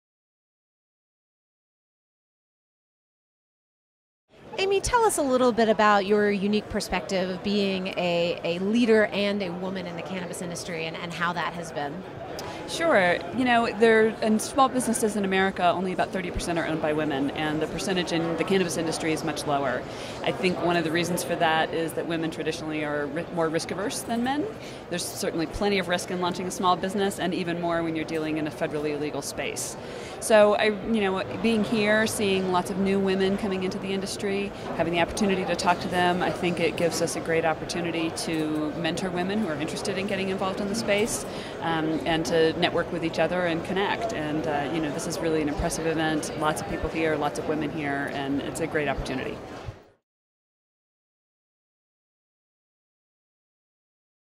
Amy Poinsett, Co-founder and CEO, MJ Freeway
Amy Poinsett Co-founder and CEO of MJ Freeway discusses her role as a leader and woman in the Cannabis industry. Cannabis Business Summit, June 2014.